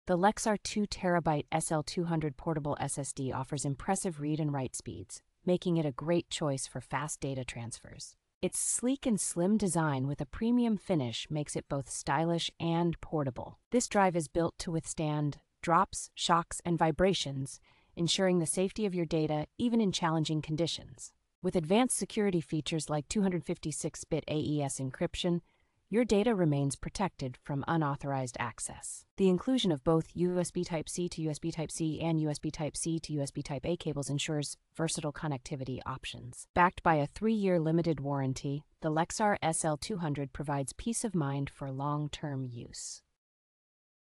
REVIEW (2024): Lexar 2TB SL200 Portable SSD. ESSENTIAL details.
The Lexar 2 Terabyte SL200 Portable SSD offers impressive read and write speeds, making it a great choice for fast data transfers.

Its sleek and slim design with a premium finish makes it both stylish and portable.

This drive is built to withstand drops, shocks, and vibrations, ensuring the safety of your data even in challenging conditions.

With advanced security features like 256-bit AES encryption, your data remains protected from unauthorized access.

The inclusion of both USB Type-C to USB Type-C and USB Type-C to USB Type-A cables ensures versatile connectivity options.

Backed by a three-year limited warranty, the Lexar SL200 provides peace of mind for long-term use.